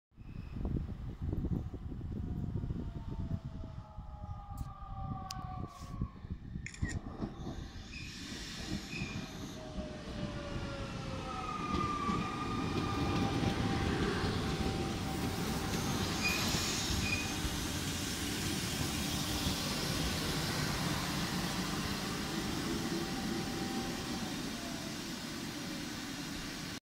MINUETTO IN ARRIVO
Reg. 3749 Paola-Reggio Calabria, delle ore 11:43,  in arrivo a Scilla FS